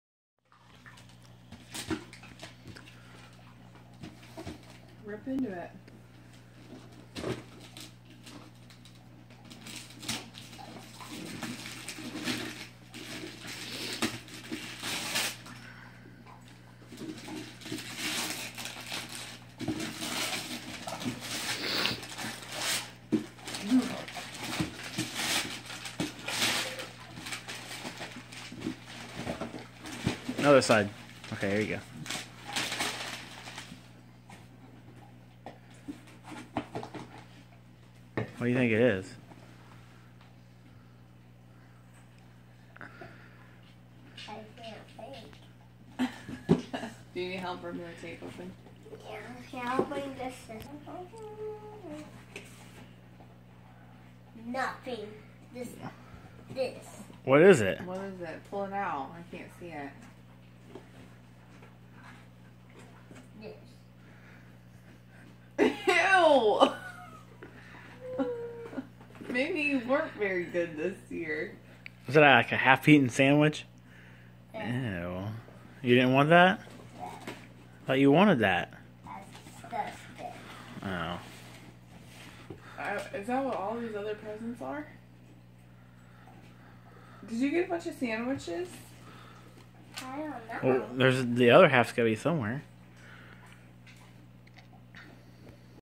Christmas Sandwich!!!
Got my son a half eaten sandwich for christmas is reaction priceless!!